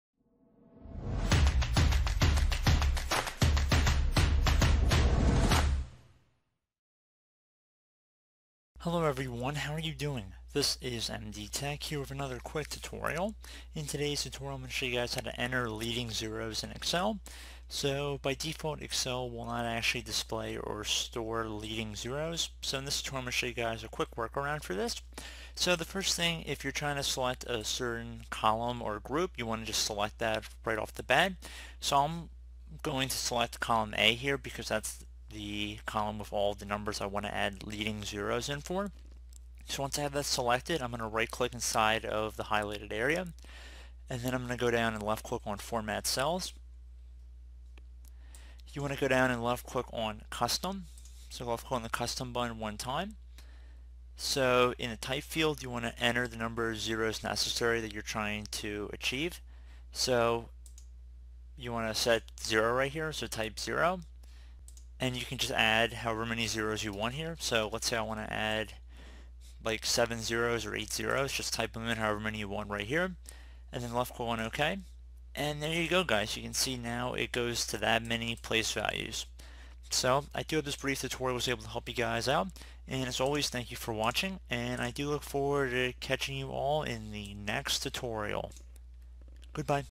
How to Format Numbers with Leading Zeros in Excel [Tutorial]
How to Format Numbers with Leading Zeros in Excel.

Leading zeros are those pesky place-holding 0 digits at the beginning of a number. Unfortunately, when you enter such a value, Excel applies the General numeric format and tosses out the leading zero. Excel won't display or store the leading 0 digit (see the Formula bar).